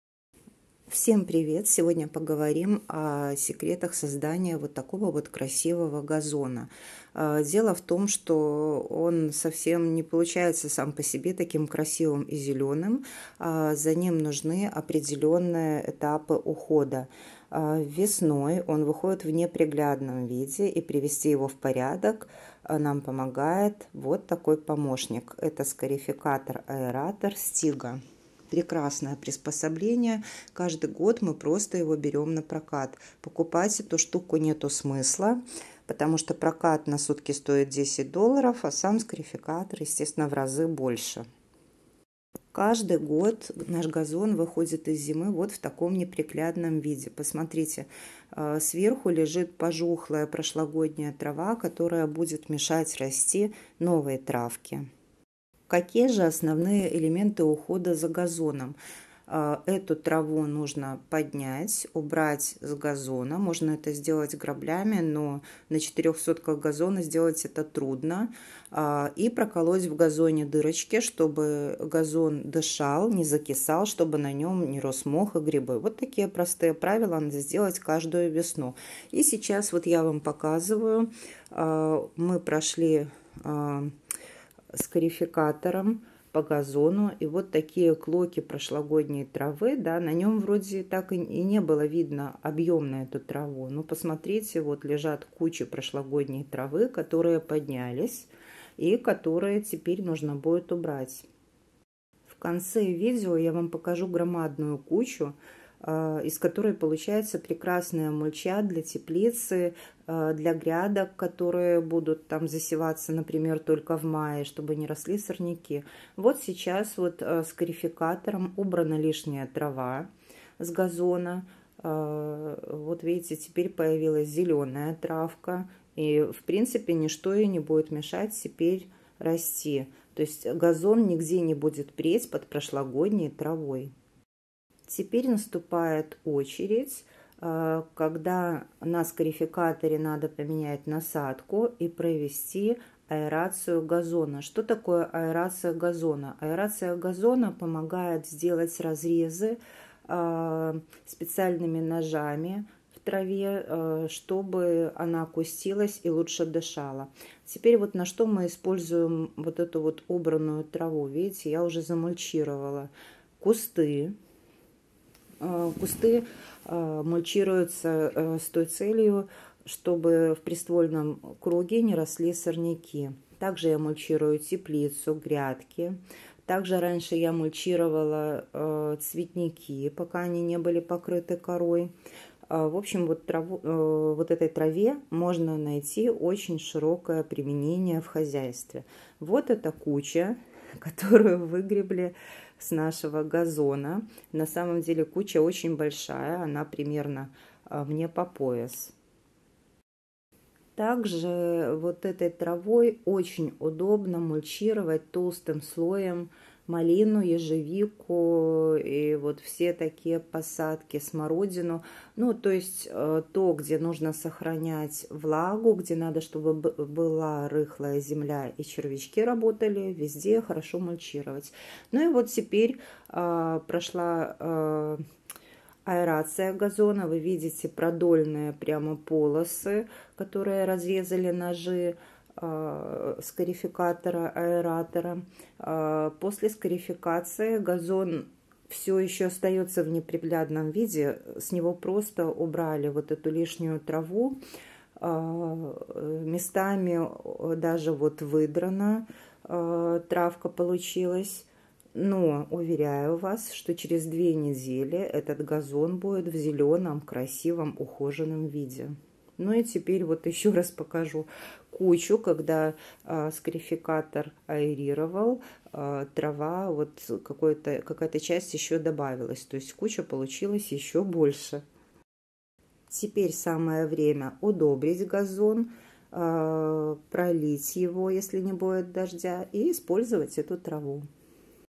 Красивый газон. Весенний уход
Весенний уход за газоном.
Когда думаешь о загородном отдыхе, то первым делом представляется чудесной красоты английский газон!

Такой мы себе и завели после окончания стройки. Прекрасный, изумрудный, пружинящий под ногами, газон из долговечного мятлика.  И чем старше, тем он прекраснее.

 И на самом деле забот он особо не требует. Но все же некоторые мероприятия проводить приходится. Ведь из зимы, к сожалению газоны выходят совсем некрасивыми.  И чем гуще и качественней был газон, тем больше будет отмершей травы...

Ставим ❤️ или пишем комментарий, чтобы я понимала, что тема вам интересна.

Первые два пункта делаем 1 раз в год. Их можно выполнять вручную, а можно с помощью скарификатора-аэратора, который совсем незадорого берется напрокат.

Первый этап: весенняя обработка.

1️⃣Через неделю-другую, как только земля подсохнет, берем грабли в руки и вычесываем старую траву, чтобы могла расти новая.

2️⃣После прокалываем вилами дырочки в земле по 8-10 уколов на метр. Это чтобы газон не закисал, дышал и росли новые кустики травы. А некоторые одевают тапки с гвоздями и ходят по газону в них.
Все это трудоёмко, поэтому аренда скарификатора-аэратора вам в помощь.

Следующий этап: подкормки:

3️⃣ Качественный газон- необычайно прожорливый и сильнопьющий товарищ 😂😂😂. Поэтому весной запасаемся любым азотным удобрением (я использую карбамид или ещё оно называется мочевиной) и перед дождем раскидываем его по норме на упаковке. Если дождя не предвидится- поливаем после удобрения. Оставлять удобрение без полива на газоне нельзя!
4️⃣ И в течение лета еще два раза после стрижки кормим газон полными (это где есть азот, фосфор, калий) удобрениями по сезону, разбрасывая их перед дождем.

5️⃣Поливаем, если долго нет дождей. В средней полосе, как правило, достаточное количество осадков, поэтому системы полива делают не часто.

Ну и не забываем протравливать 1-2 раза за лето газон от сорняков.

☝🏻☝🏻☝🏻Это и есть рецепт наикрасивейшего газона!!! Правда не трудно?